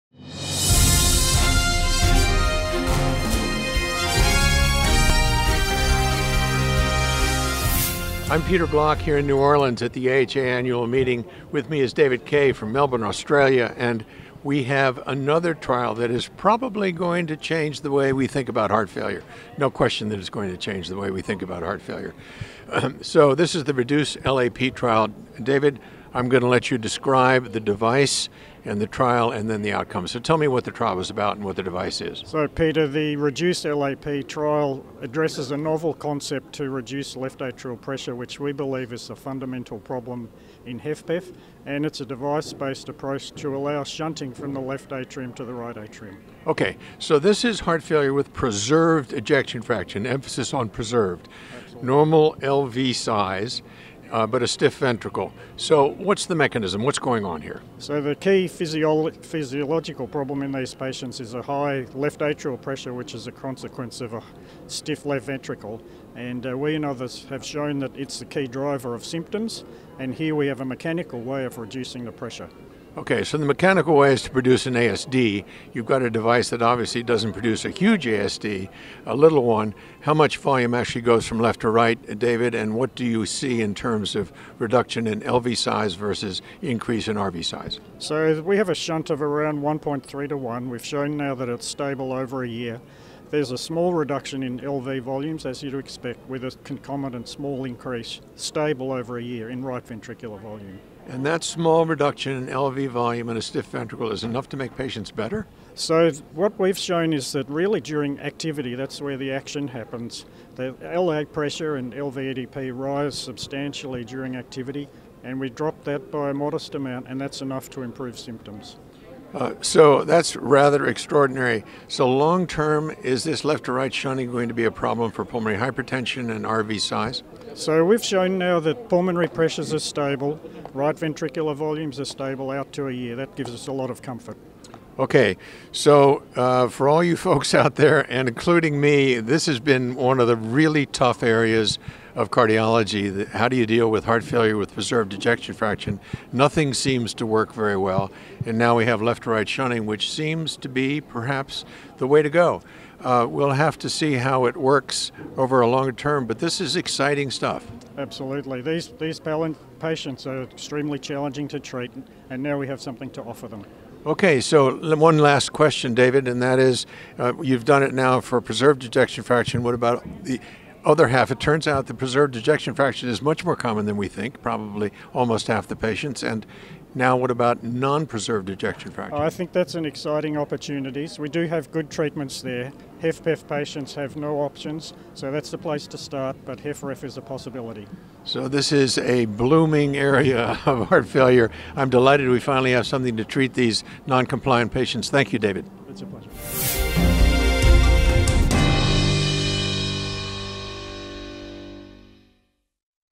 AHA 2016 | REDUCE LAP HF
Dr. Peter Block interviews Dr. David Kaye on the results REDUCE LAP HF trial to assess the impact of the IASD in patients with HFPEF.